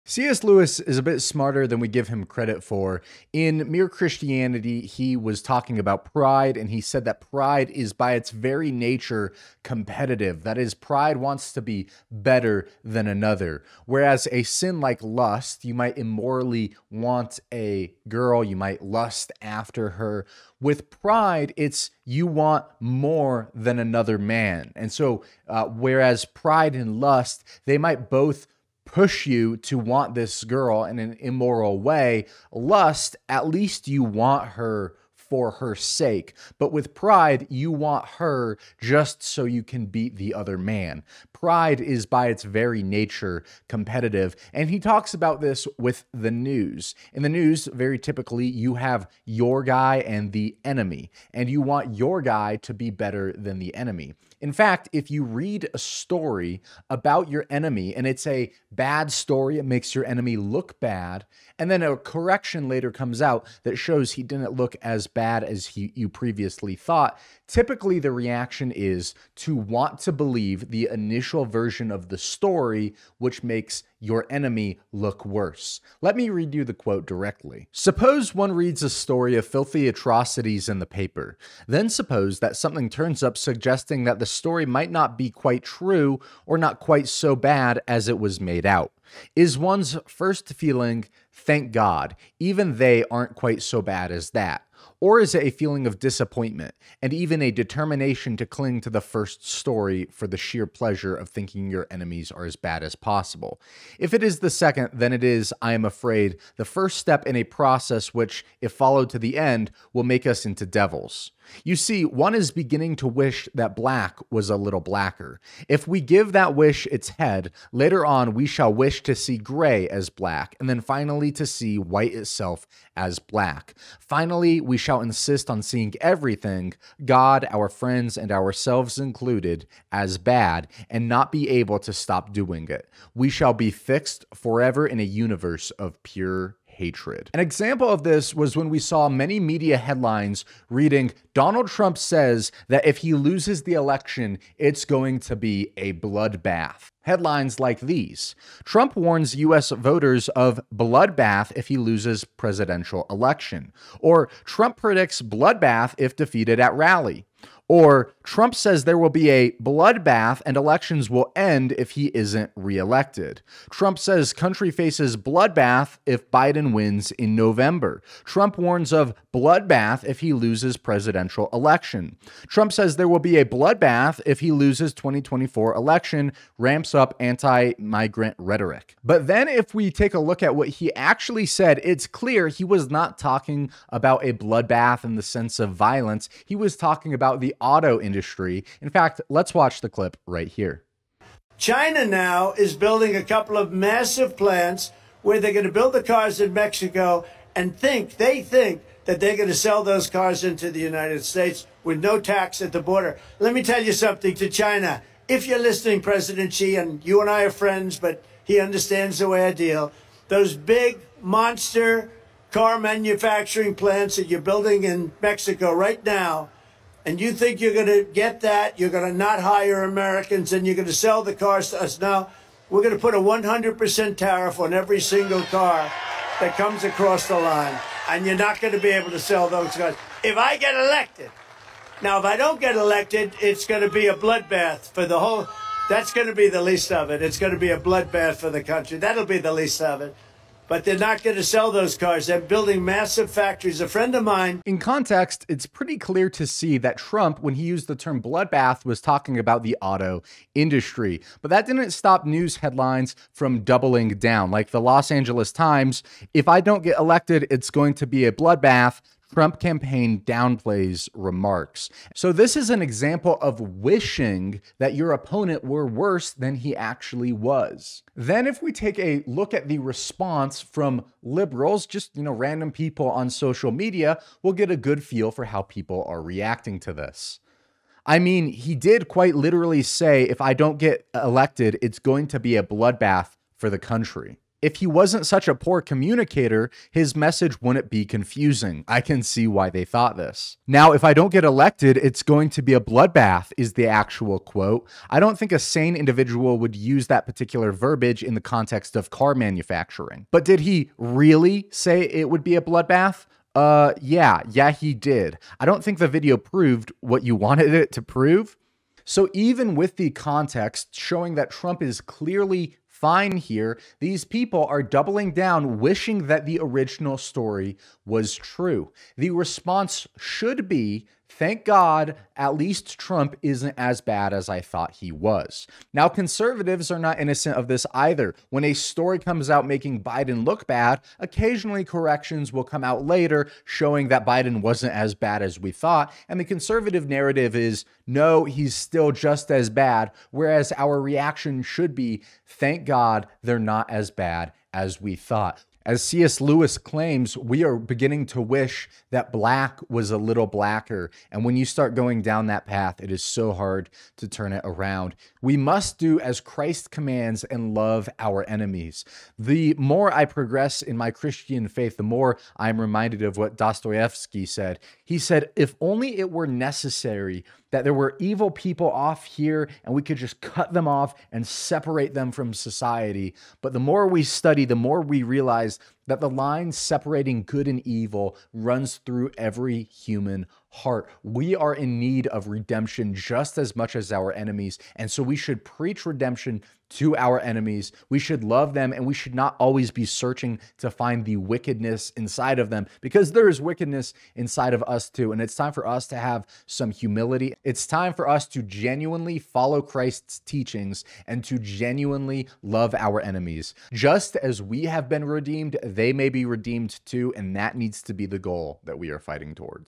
How C.S. Lewis PREDICTED 2025 Headlines | We NEED To Know What He Said
Think you know all there is to know about C.S. Lewis? Think again. In this video, we dive into some of his powerful insights on pride, forgiveness, and the dangers of wishing sin upon our enemies. Watching this video, you'll be challenged to genuinely love your enemy, rather than wishing that black were a little blacker.

00:00 The Brilliance of C.S. Lewis
02:16 An example of Lewis' warning
05:18 Loving your enemies is the solution